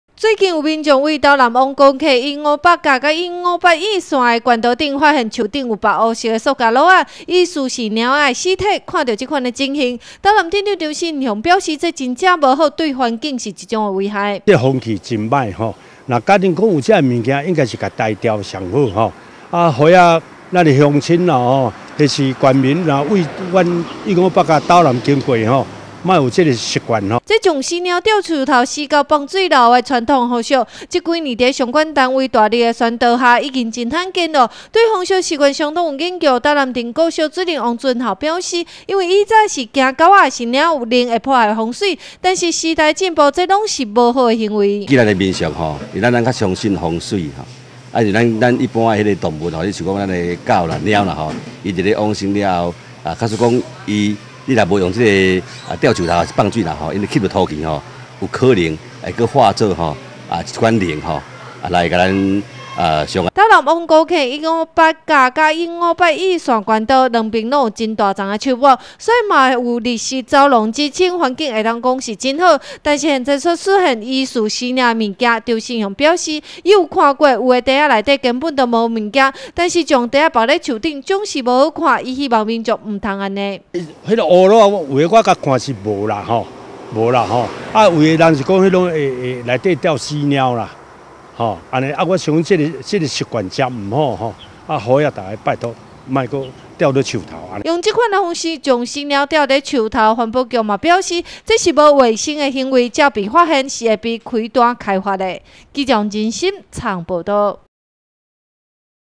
雲林新聞網-斗南158乙縣道發現死貓吊樹頭
傳統風俗中有死貓吊樹頭、死狗放水流，不過這種情形，在現在已經很罕見，但是最近有民眾發現在斗南鎮往古坑鄉的158甲跟乙線雲林縣道上，樹頭有吊著的黑色塑膠袋，疑似是貓的屍體，斗南鎮長張勝雄並提出呼籲，希望大家要維護環境。【以上新聞可從哈TV App收視】